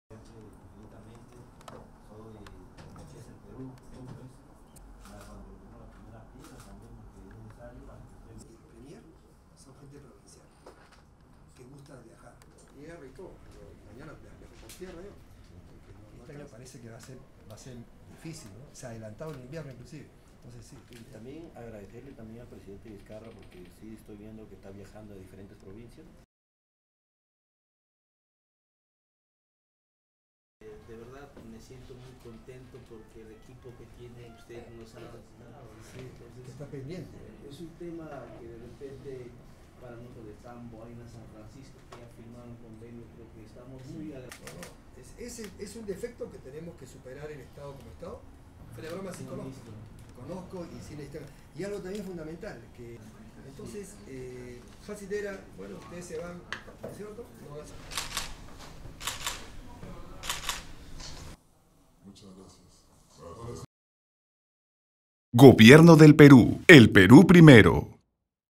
Ministro del Interior se reúne con alcaldes y se suscriben convenios
Subscríbete a nuestro canal y recibe toda la información de las actividades y noticias más importantes de nuestro sector.